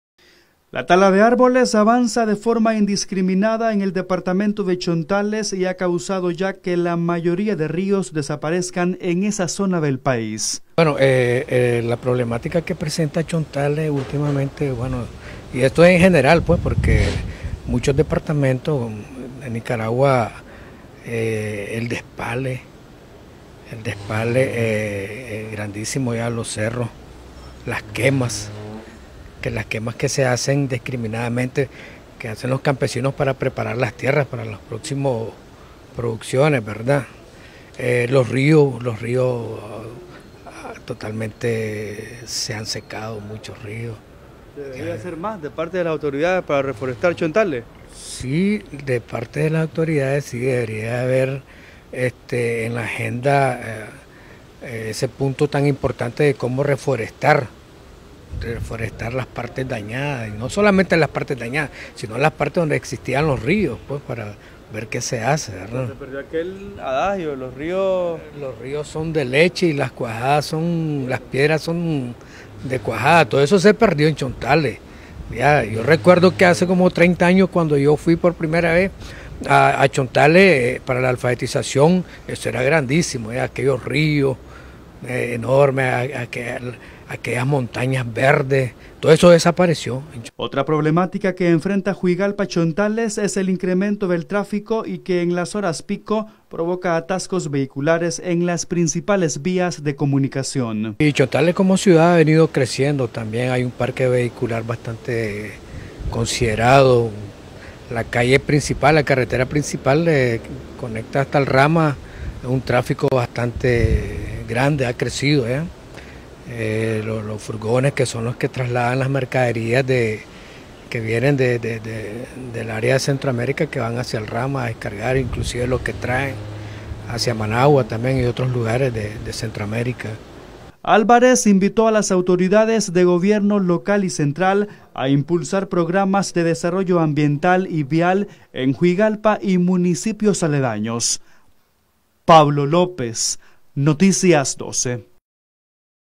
El Departamento de Chontales era conocido por la Frase Ríos de Leche y Piedras de Cuajadas.
El Departamento de Chontales era conocido por la Frase Ríos de Leche y Piedras de Cuajadas, pero la desaparición de Ríos, hace que esa Frase sea solo cosa del pasado.